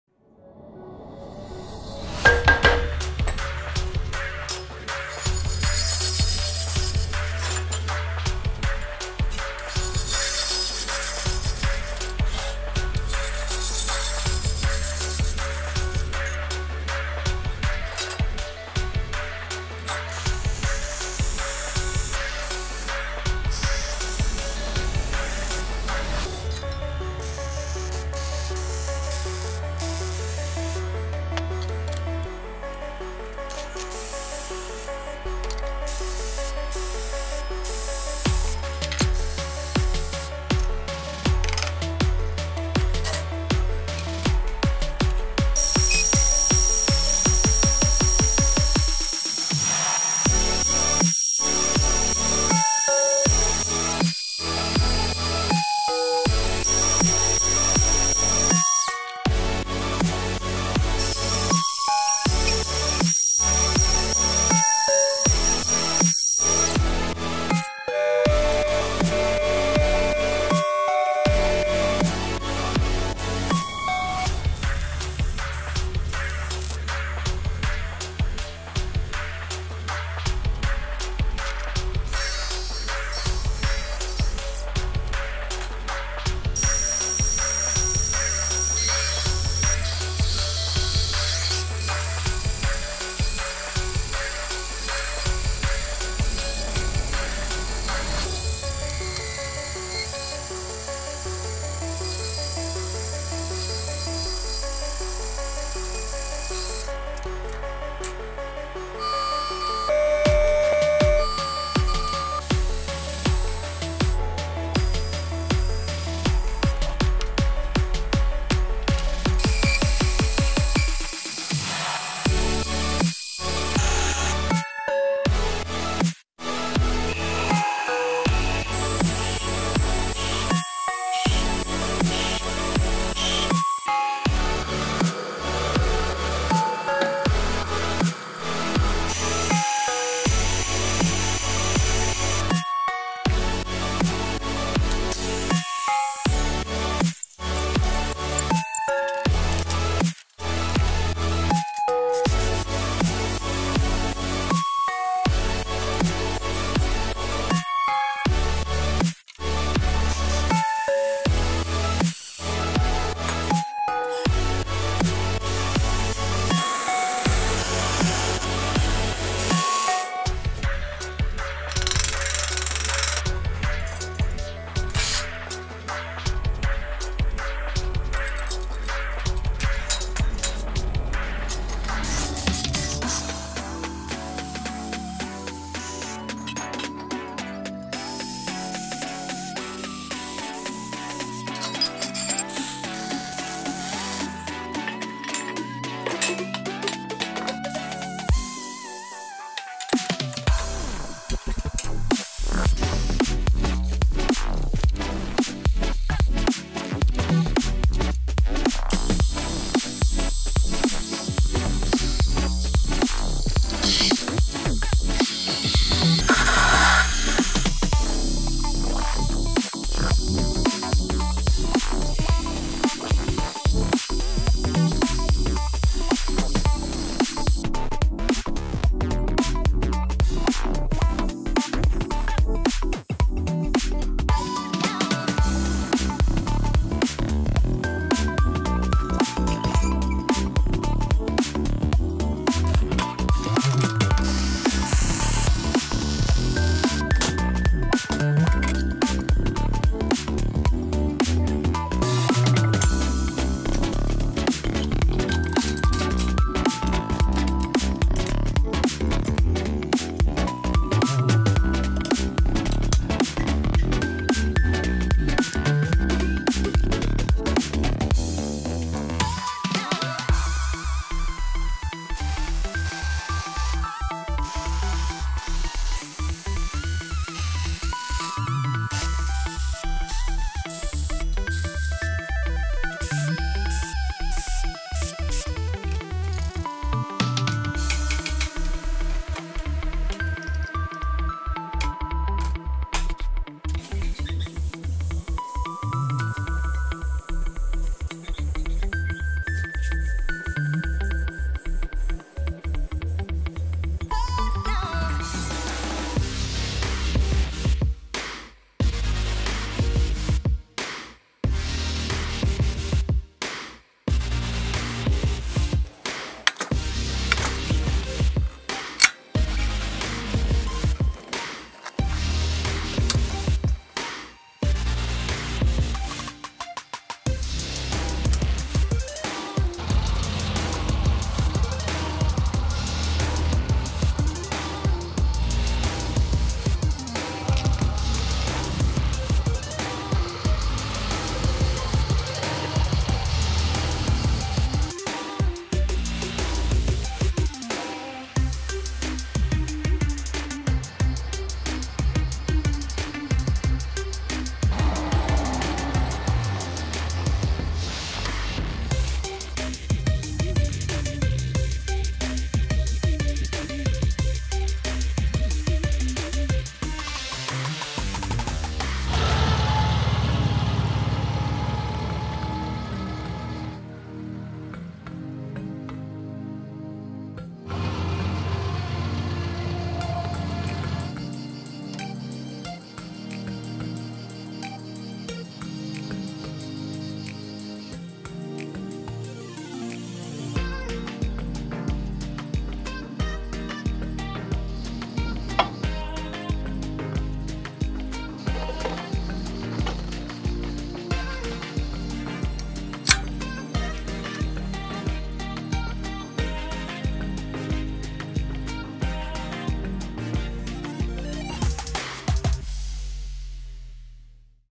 Make it Extreme's Motorbike Trailer
In this video we present the construction of a trolley to transfer the motorbike. It is an essential construction for the ones who have a motorbike and they need to transfer it on their car to rally tracks or to travel long distances by using the car!

► Tools we recommend:
Engineers Squares Set
25FT Tape Measure